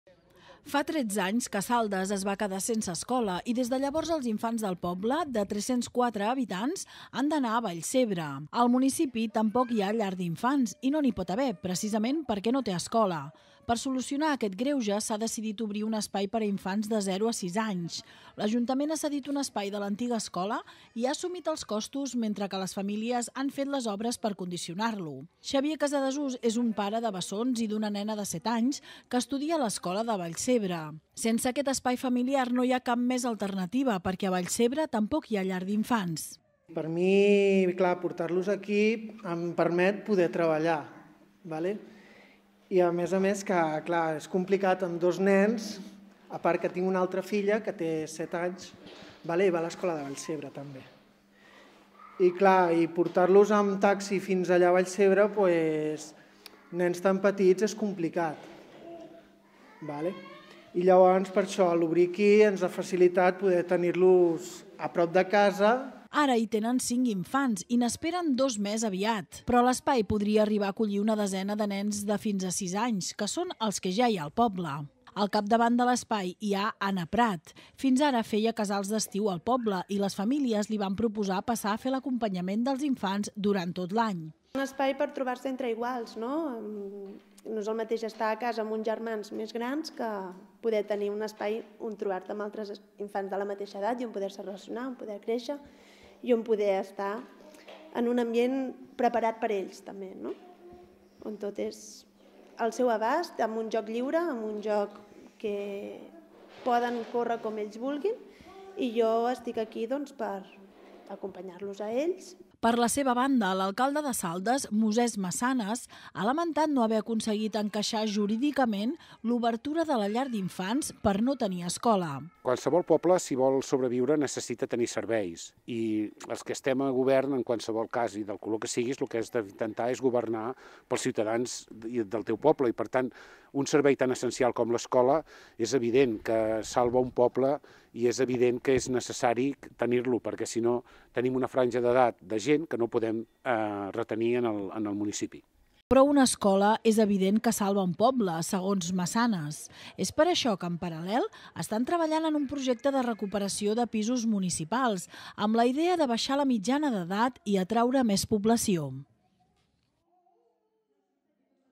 Saldes obre un nou espai per a infants de 0 a 6 anys
Fa 13 anys que Saldes es va quedar sense escola i, des de llavors, els infants del poble, de 304 habitants han d'anar a Vallcebre. Al municipi tampoc hi ha llar d'infants i no n'hi pot haver, precisament, perquè no té escola. Per solucionar aquest greuge, s'ha decidit obrir un espai per a infants de 0 a 6 anys. L'Ajuntament ha cedit un espai de l'antiga escola i ha assumit els costos mentre que les famílies han fet les obres per condicionar-lo. "
 Xavier Casadessús és un pare de bessons i una nena de 7 anys que estudia a l'escola de Vallcebre. Sense aquest espai familiar, no hi ha cap més alternativa perquè a Vallcebre tampoc hi ha llar d'infants.
 Ara hi tenen cinc infants i n'esperen dos més aviat. Però l’espai podria arribar a acollir una desena de nens de fins a 6 anys que són els que ja hi ha al poble.
 Al capdavant de l'espai hi ha Anna Prat. Fins ara feia casal d'estiu al poble i les famílies li van proposar passar a fer l'acompanyament dels infants durant tot l'any.
 Per la seva banda, l'alcalde de Saldes, Mosès Masanas, ha lamentat no haver aconseguit "encaixar" jurídicament l'obertura de la llar d'infants per no tenir escola
 Però una escola "és evident que salva un poble", diu. És per això que, en paral·lel, estan treballant en un projecte de recuperació de pisos municipals amb la idea de baixar la mitjana d'edat i atraure més població.